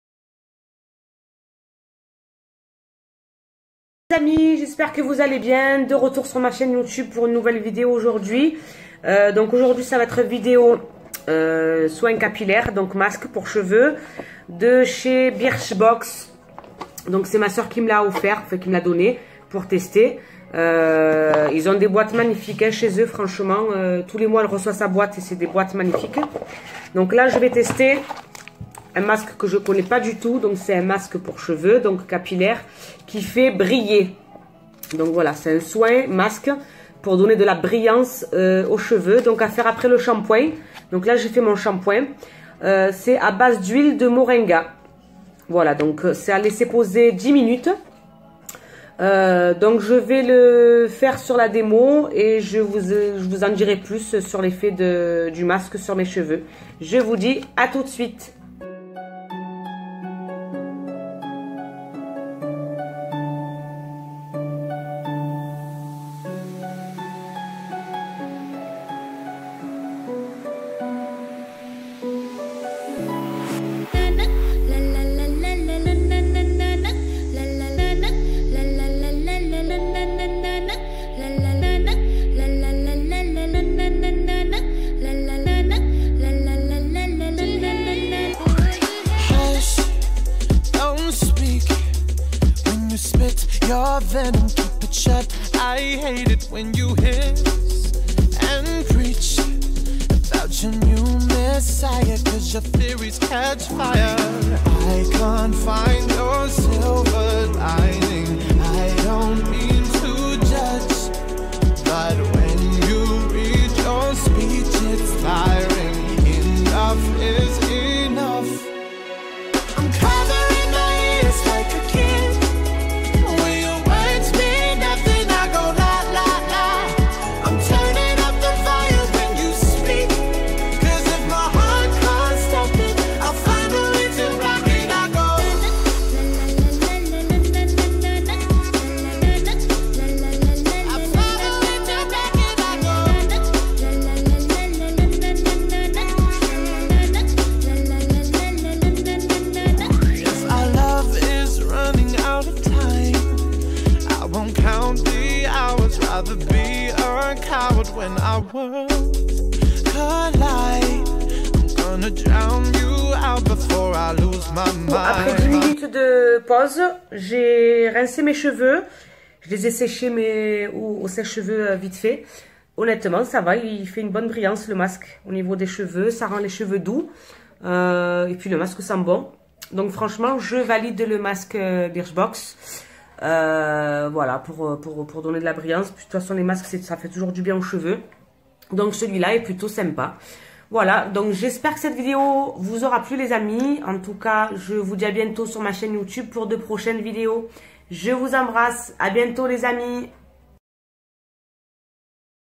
BIRCHBOX JE TESTE MASQUE CAPILLAIRE
Il faut laisser poser 10mn rincer puis sécher au sèche cheveux.